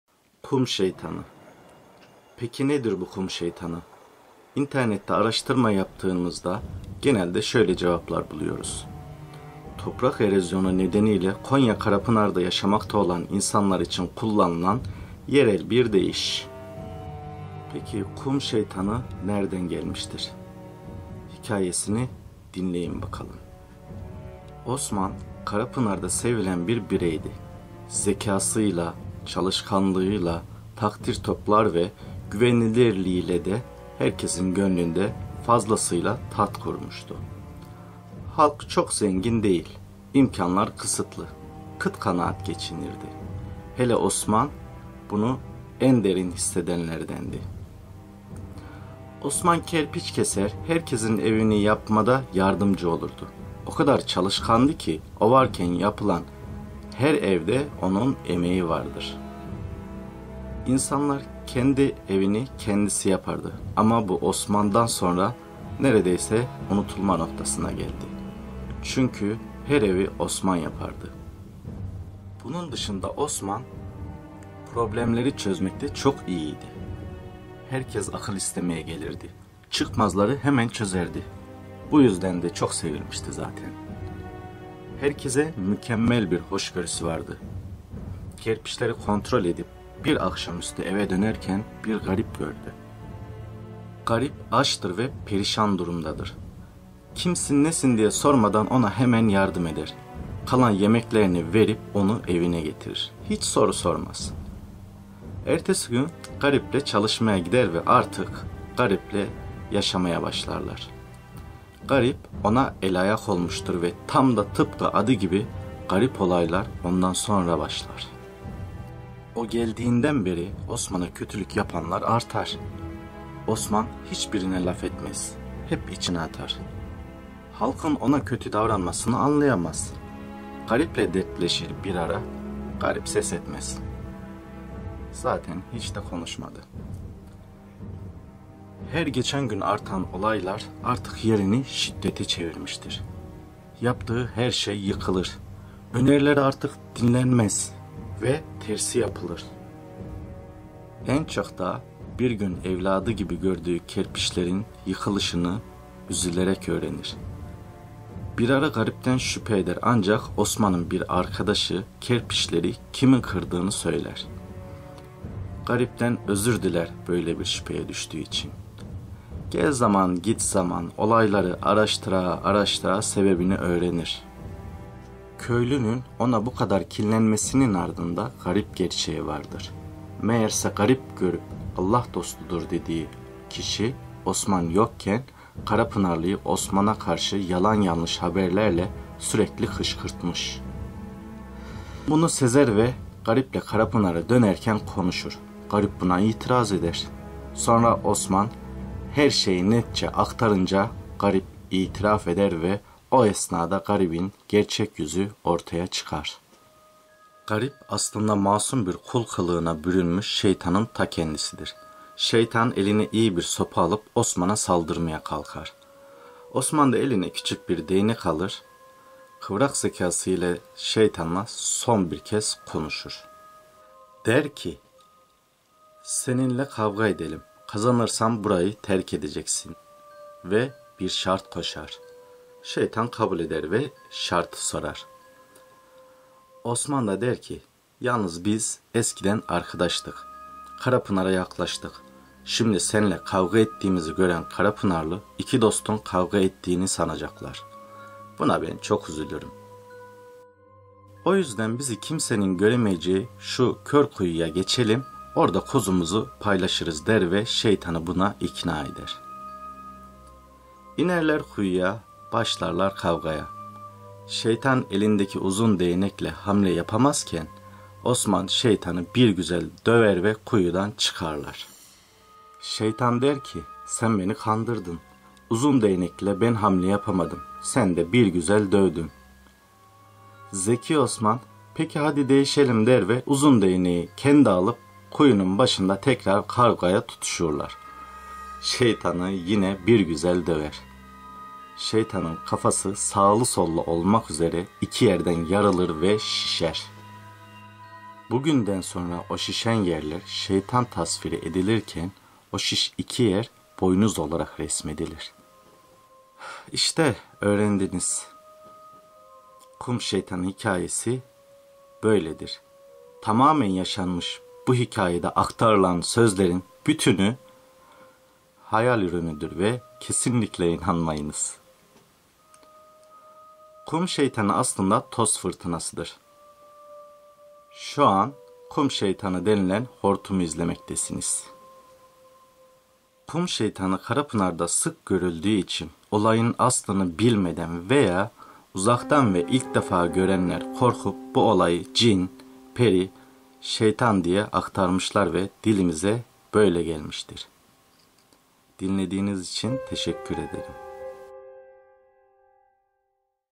KUM ŞEYTANI HİKAYESİ - KARAPINAR
Kum Şeytanı nedir? Karapınarlı'ya neden kum şeytanı denir? İşte size gerçek Hikayesi ve bütün sözler tarafımdan uydurulup, var olan hikayeye bağlanmıştır.